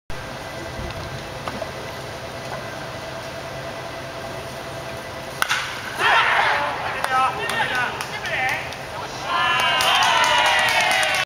【第62回JABA広島大会】広島カープ 内田 湘大のバッティングフォーム_2023082901
第62回JABA広島大会、広島東洋カープ VS JFE西日本@マツダスタジアムでの広島カープ 内田 湘大のバッティングフォームです。